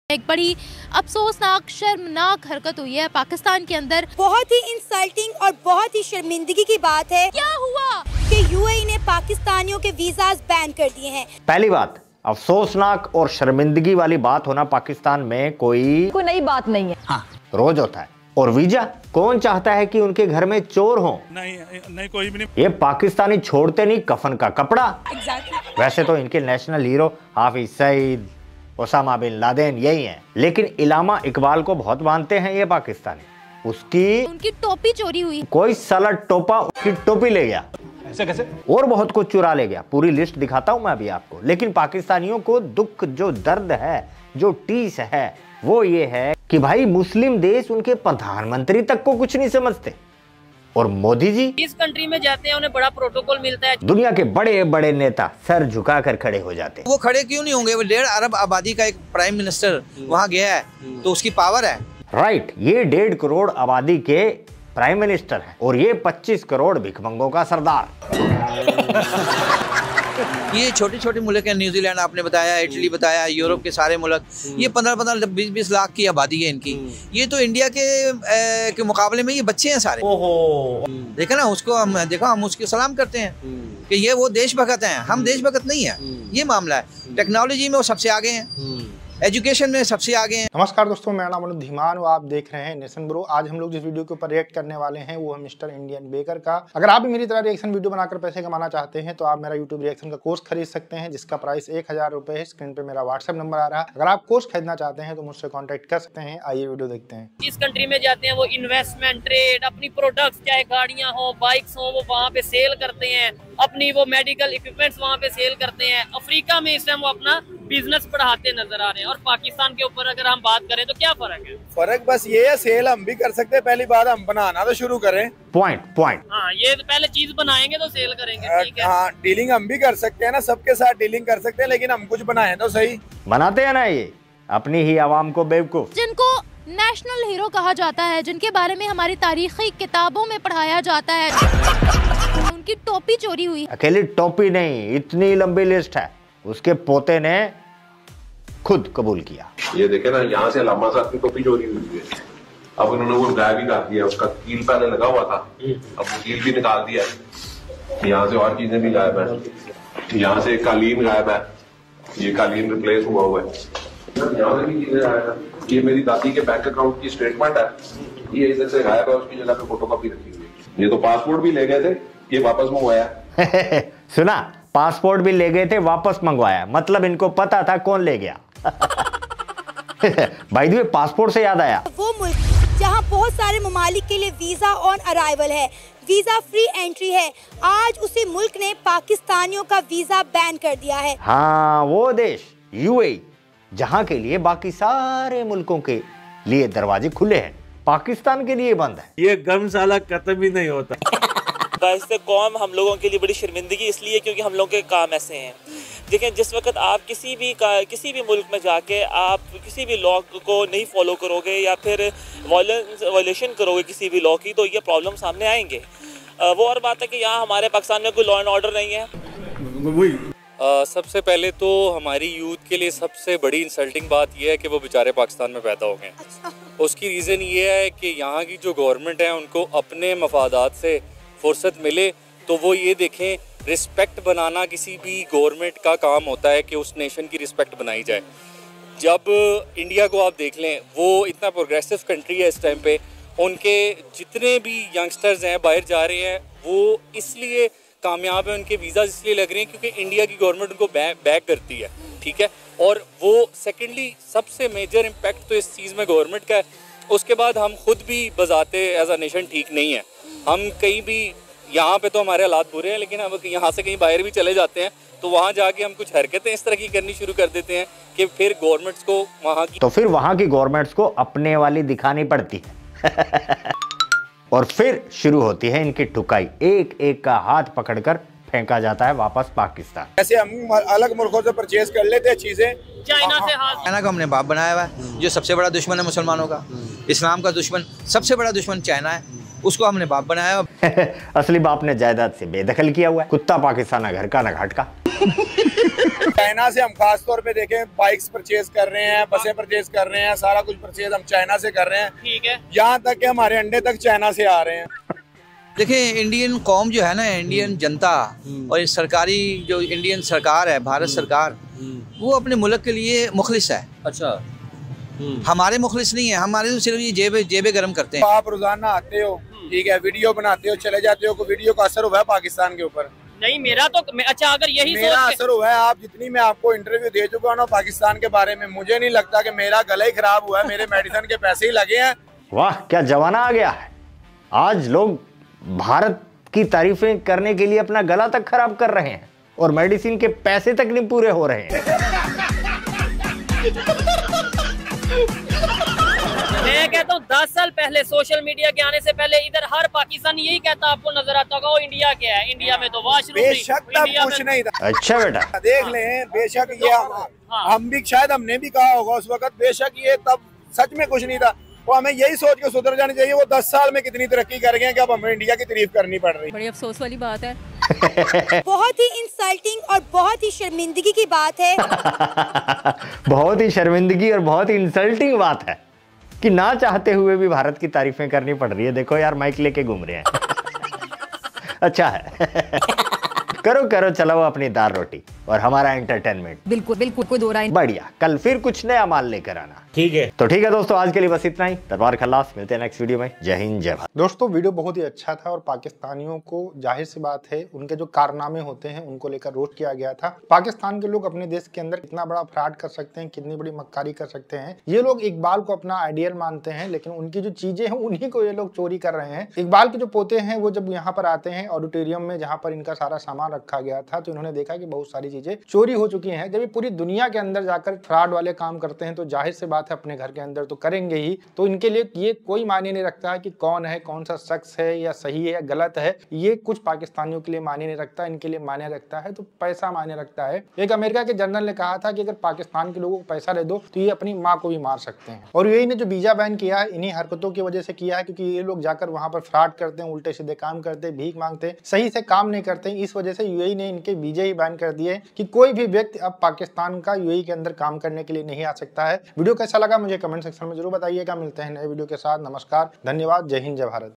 दुबई शेख ने लात मारकर पकिस्तानियों को अपने देश से निकाला ,पाकी रोए | Pak Media Crying 😭😂
दुबई शेख ने लात मारकर पकिस्तानियों को अपने देश से निकाला ,पाकी रोए
Open a Free Demat Account with Angel One

Original Video Channel Name  -